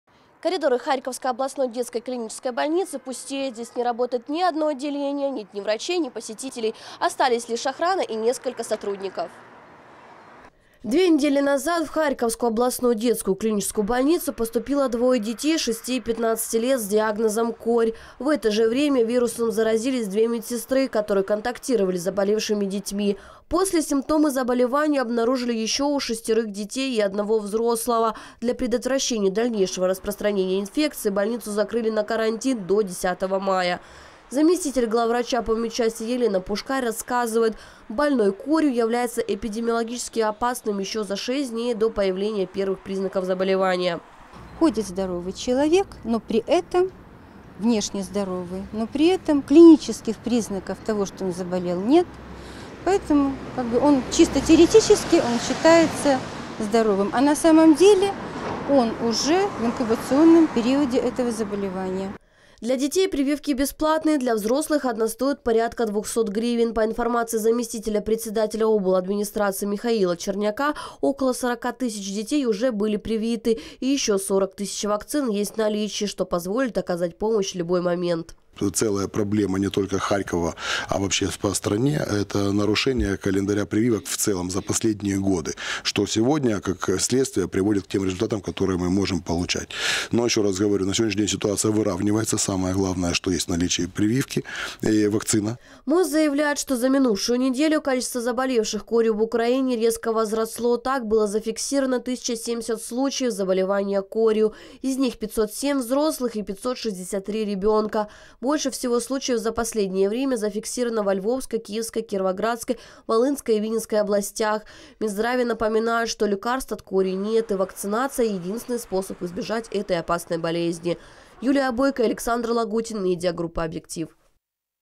Областная детская больница закрыта на карантин из-за кори
Накануне врачи обнаружили среди пациентов нескольких больных с соответствующими симптомами. Для предотвращения дальнейшего распространения инфекции больницу закрыли на карантин до 10 м